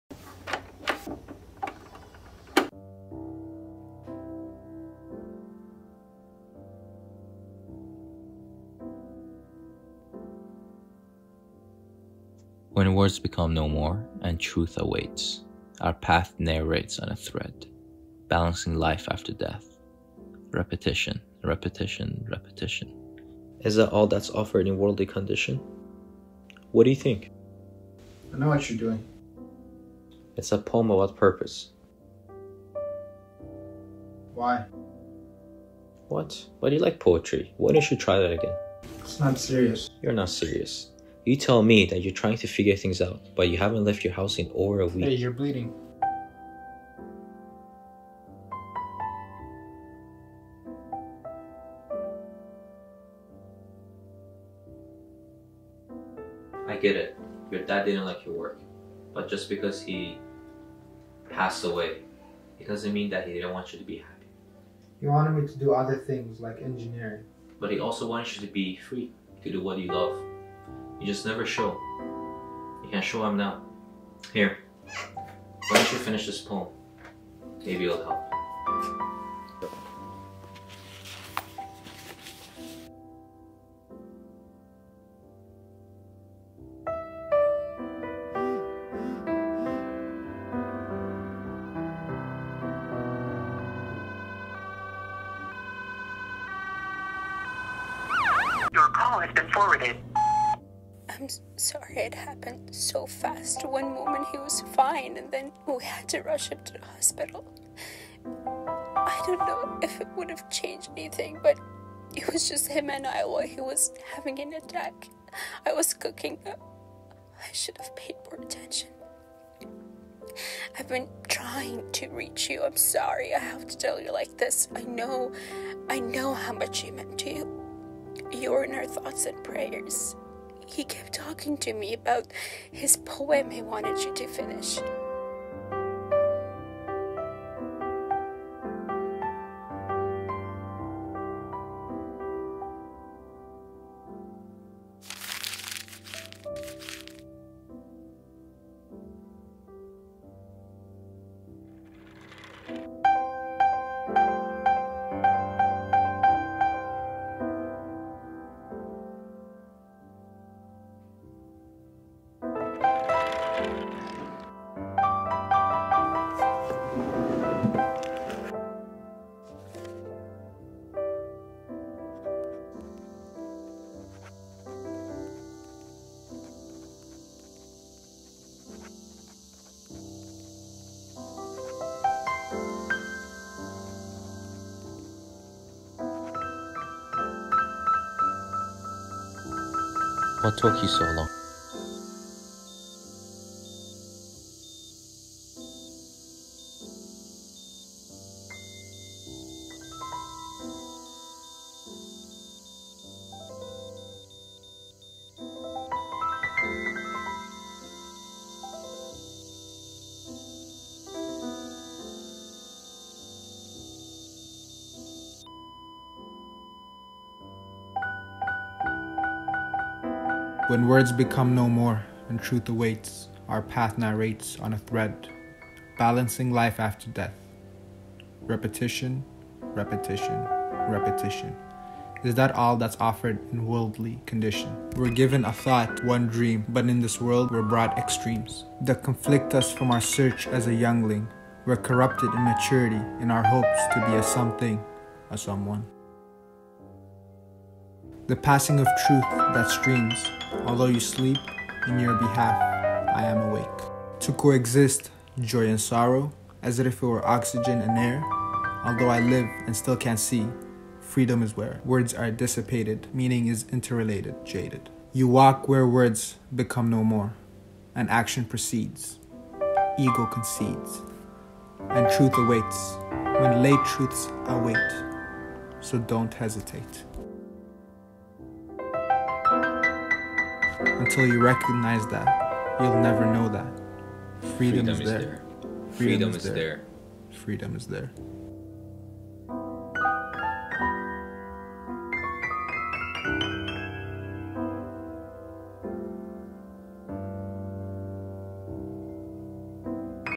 FREEVERSE A Short Film
A young man is trying to regain his love of poetry, and is assisted by his friend to reclaim his passion.

Application for the 2021 York University Film Production Program

Starring Abdul Seyyid & Parsa Hazeri
Voice Acting by Nikta Hazeri
Production Assistant and played as “feet under the bedcovers” by Travis Thakurdin

Esther Maloney
Anna Castelaz
Liam Dousti